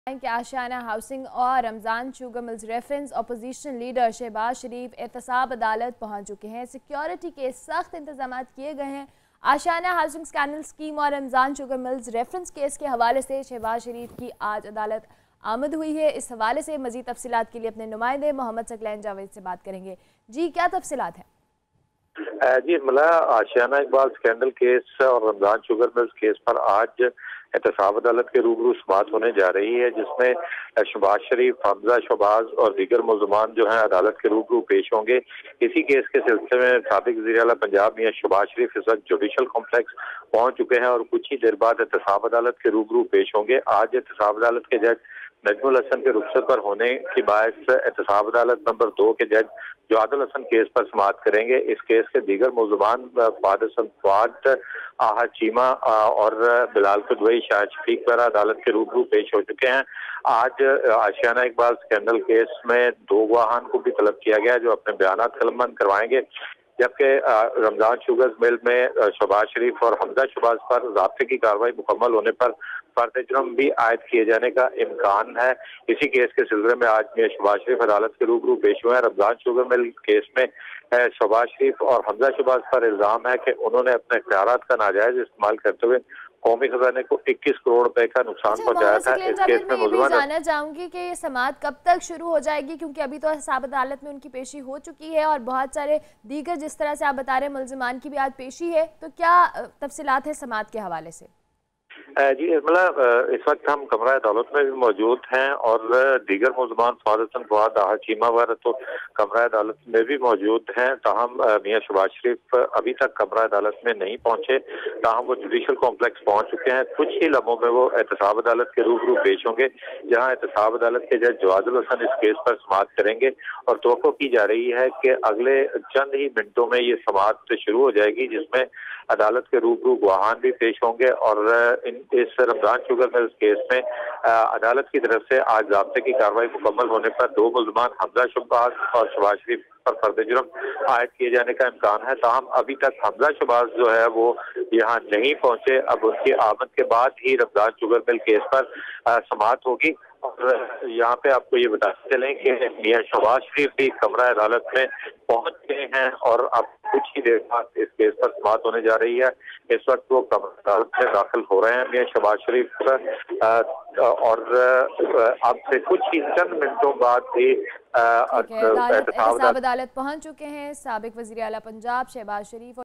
Shehbaz Sharif appears before NAB Court in Ramzan Sugar Mills reference
Shehbaz Sharif appears before NAB Court in Ramzan Sugar Mill reference